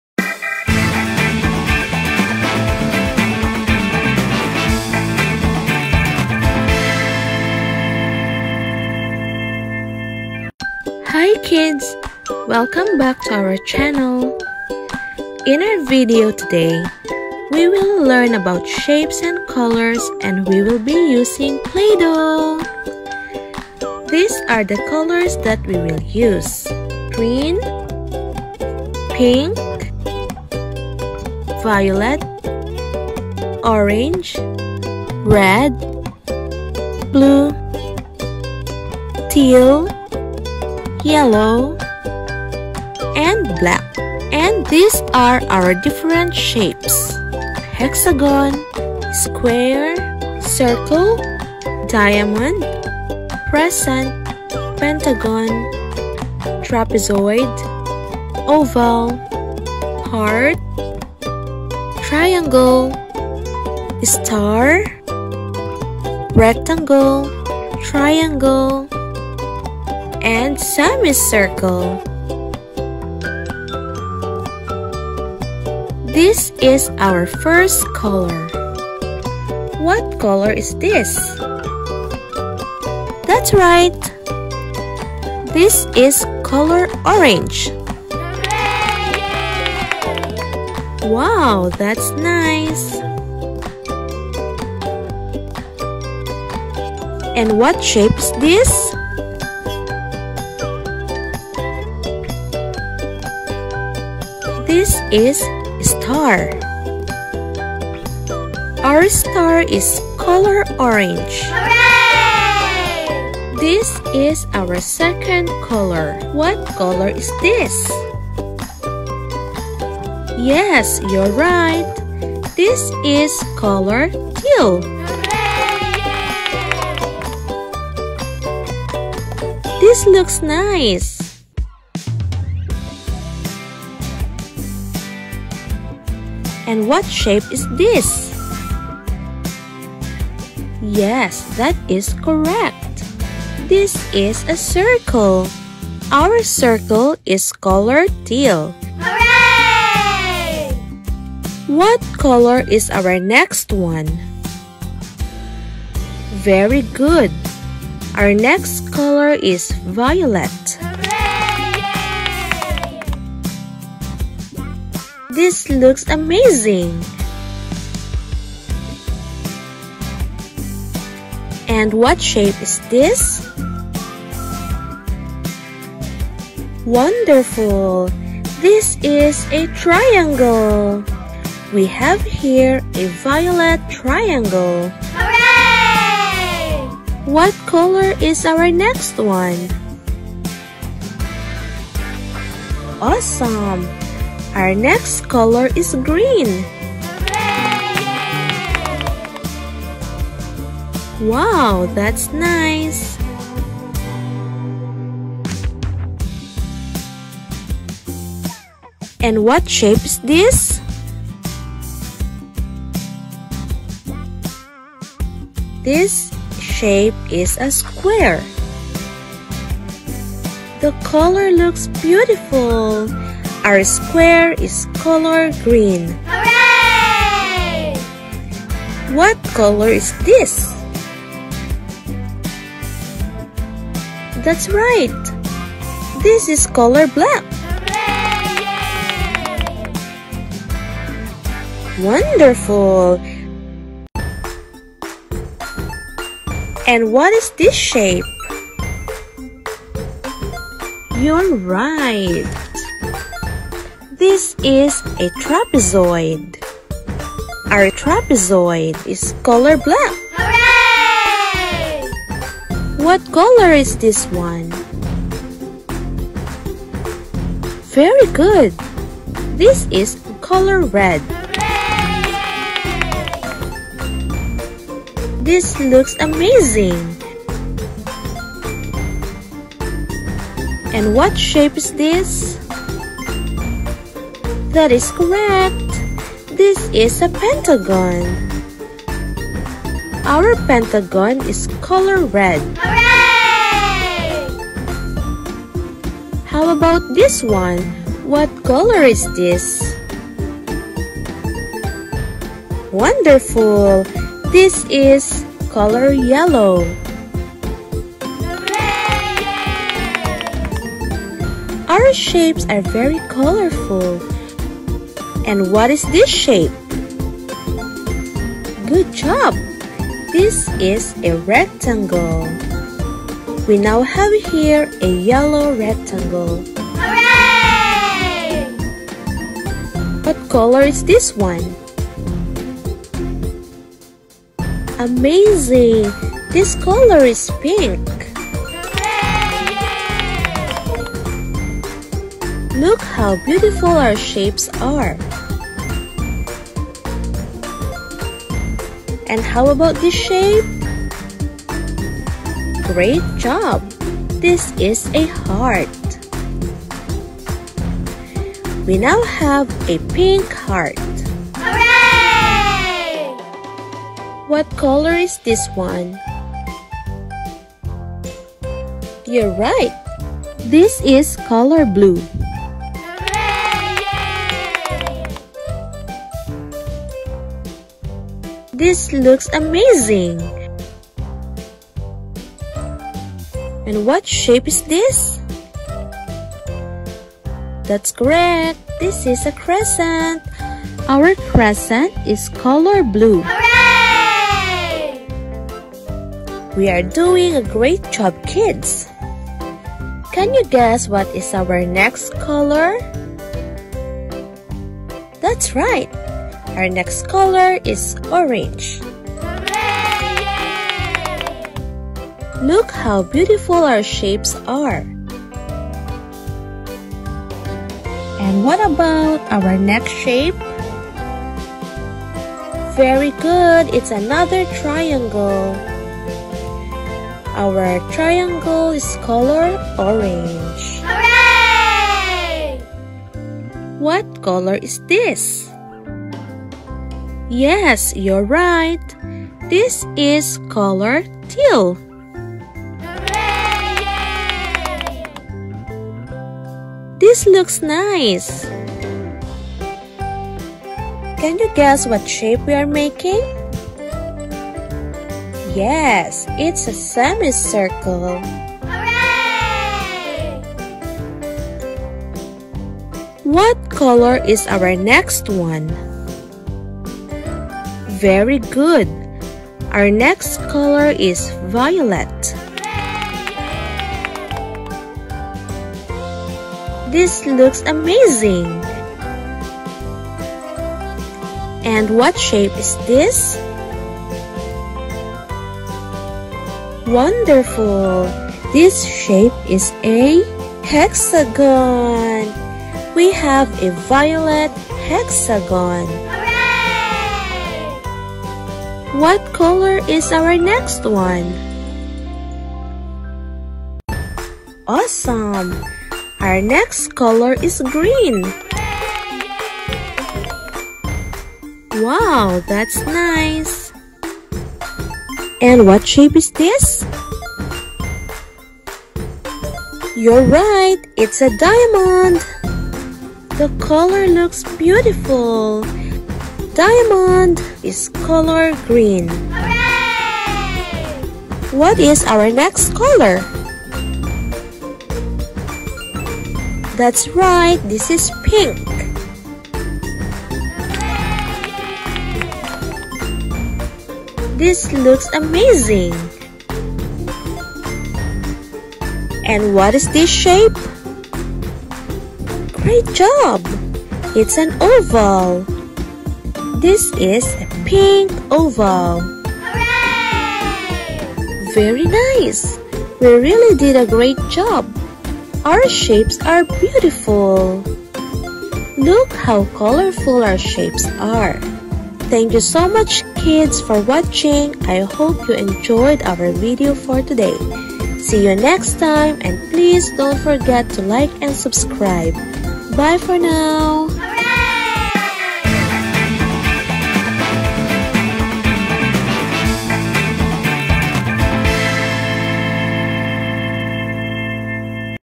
Play Doh I Learn Shapes & Colors
In this activity, children will enjoy learning shapes by using colorful play-doh's.

Children will develop their audio-visual skills by engaging them into a question & answer interaction and showing them a fun and interesting way to learn shapes, playing with play-doh!!!